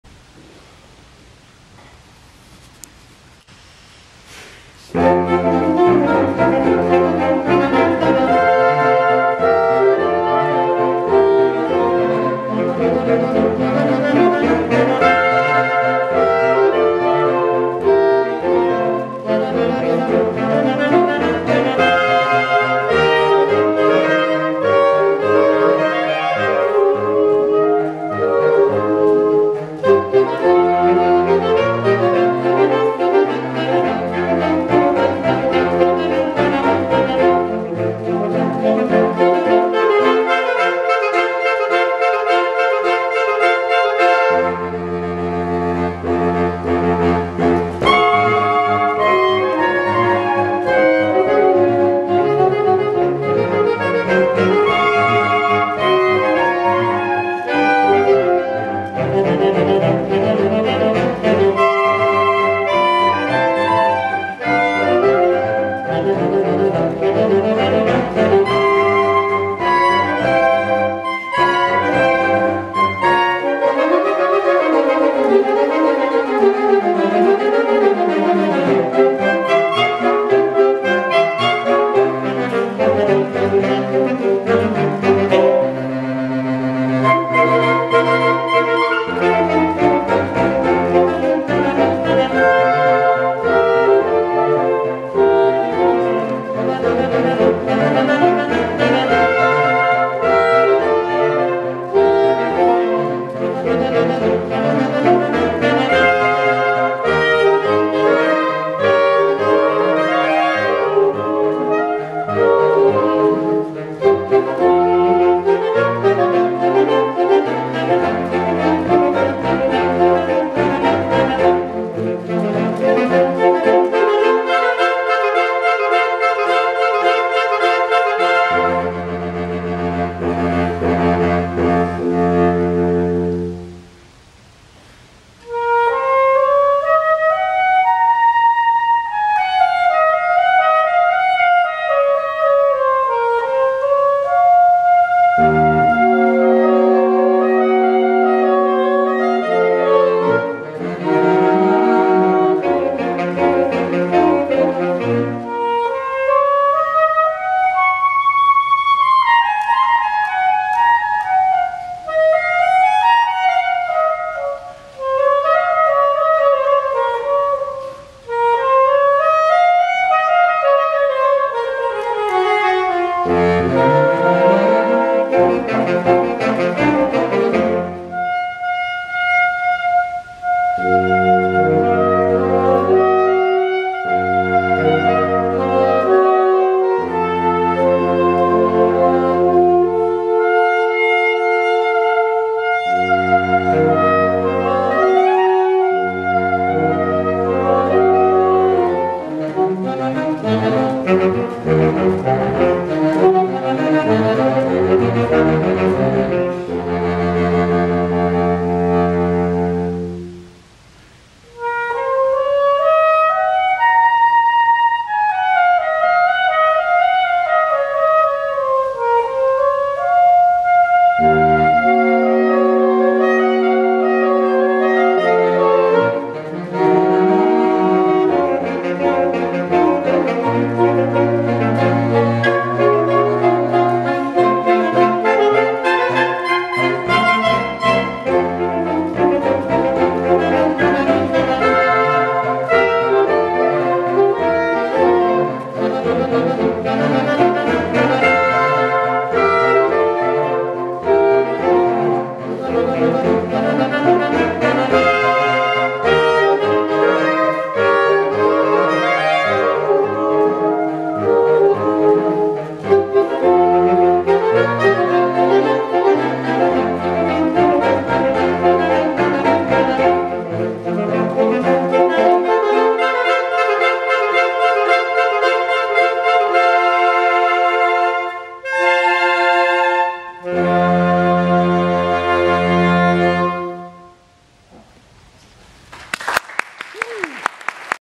University Saxophone Quartet
California State University at Long Beach, Bob Cole Conservatory of music, Saxophone Quartet video: Paul Cotton, Alex Baiseri, Krissia Molina & Michael Camacho filmed by Betty Cotton performed on April 8, 2016